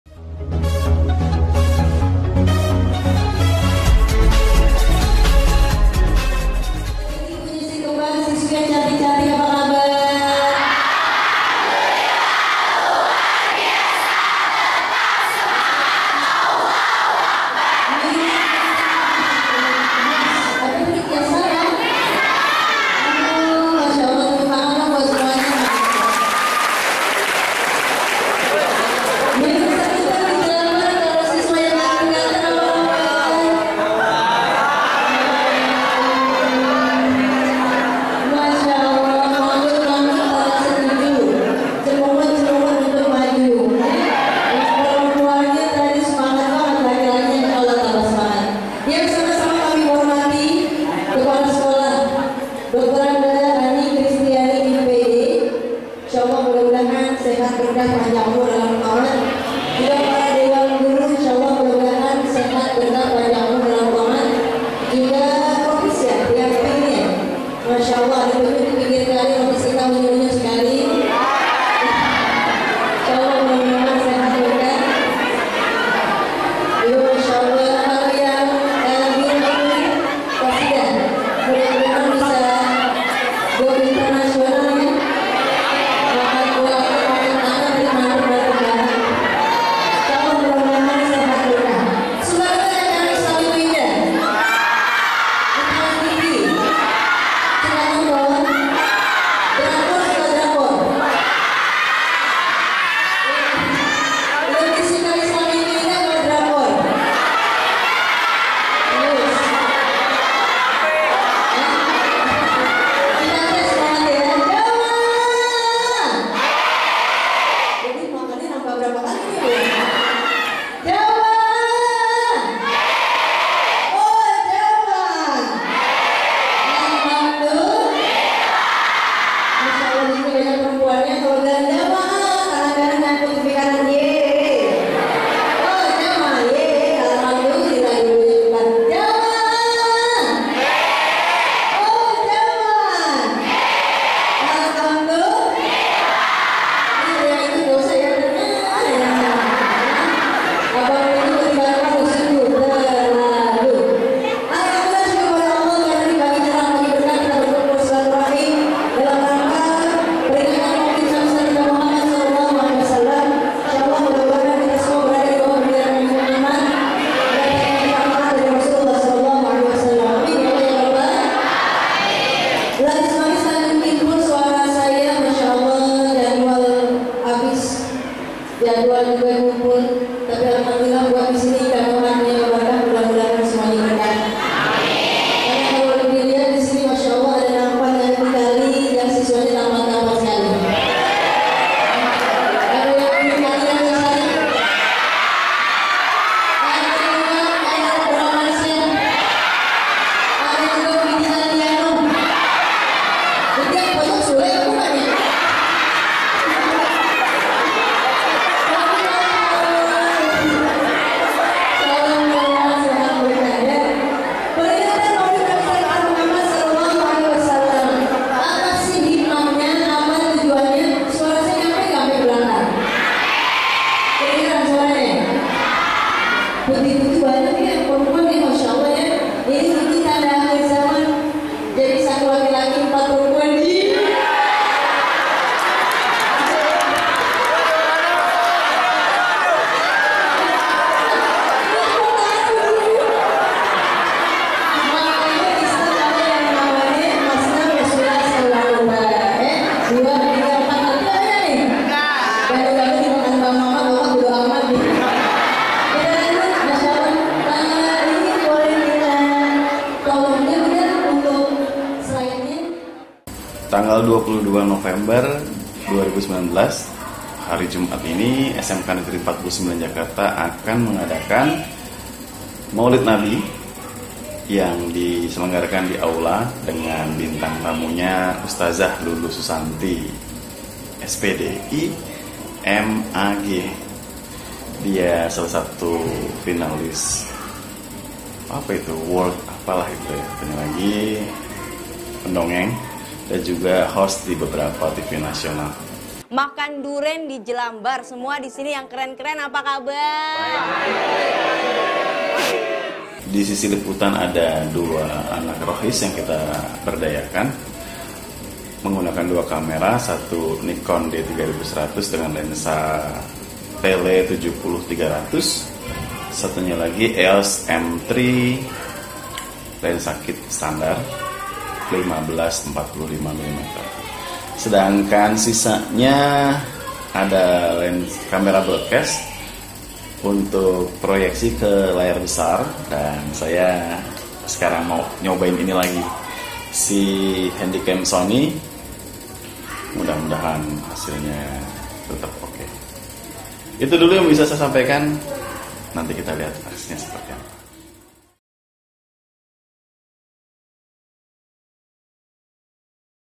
Maulid Nabi Muhammad SAW 1441 H di SMKN 49 Jakarta
Video ini masih tahap awal Pelaksanaan Maulid Nabi Muhammad dengan bintang Tamu Ustadzah Lulu Susanti, S.Pd.I., M.Ag.
Nanti yang versi full akan segera tayang dan dikabari segera ^_^